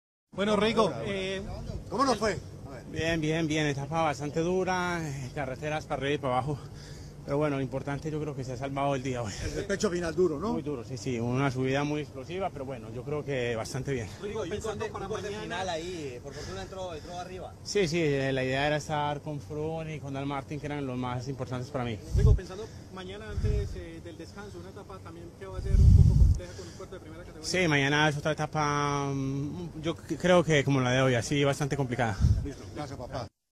Rigoberto Uran | La idea es estar con Froome | Etapa 14 Declaraciones | Tour de Francia 2017
Declaraciones Etapa 14 de Rigoberto Urán, donde afirma que ha salvado el día, y que lo importante es estar con Froome.